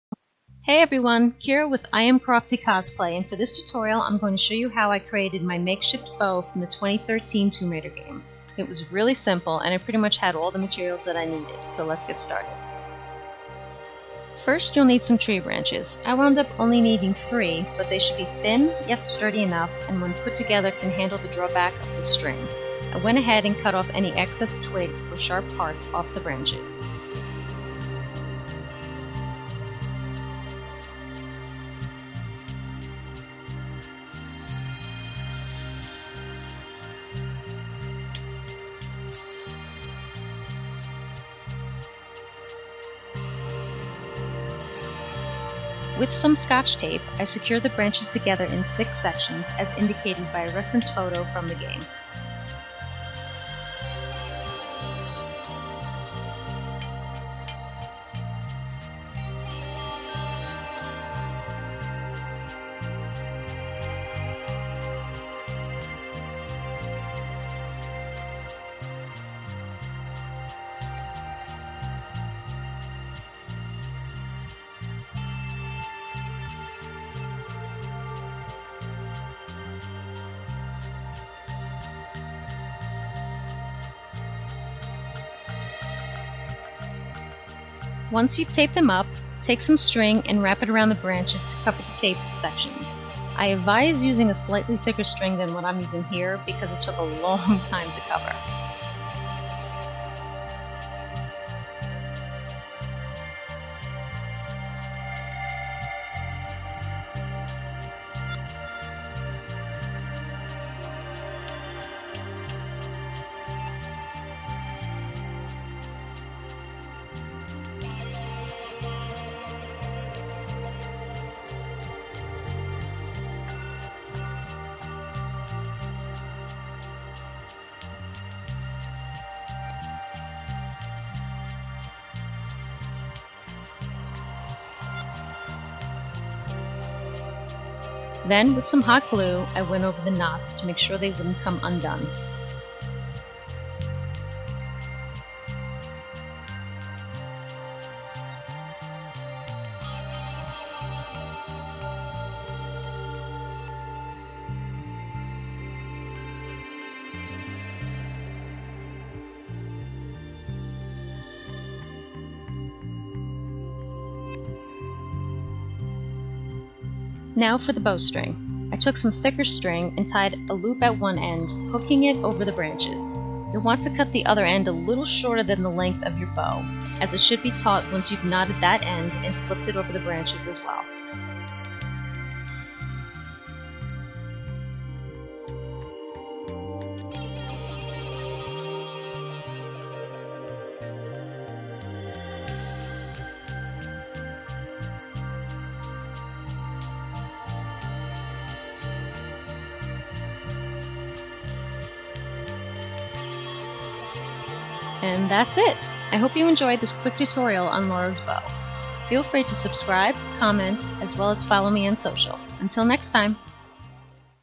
Lara Croft | Tomb Raider 2013 | Makeshift Bow Cosplay Tutorial
HQ available! In this quick tutorial, I show you how I made my makeshift bow from the 2013 Tomb Raider game. I pretty much already had everything I needed- just needed to find some tree branches!

|| MY ETSY SHOP ||